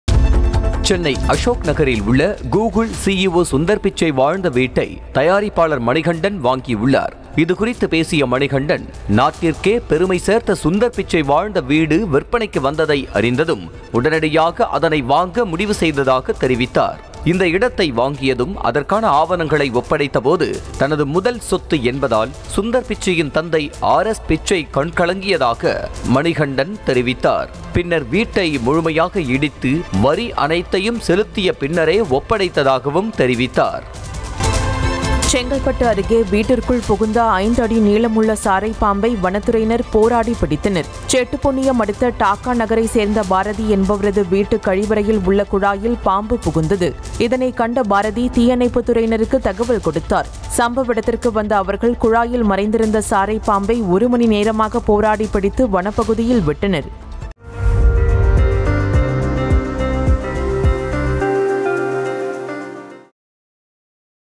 சுந்தர் பிச்சை வாழ்ந்த வீட்டை வாங்கிய தயாரிப்பாளர்.. கண்கலங்கிய ஆர்.எஸ்.பிச்சை | Sundar Pichai
சுந்தர் பிச்சை வாழ்ந்த வீட்டை வாங்கிய தயாரிப்பாளர்.. கண்கலங்கிய ஆர்.எஸ்.பிச்சை | Sundar Pichai | Producer Manikandan | RS Pichai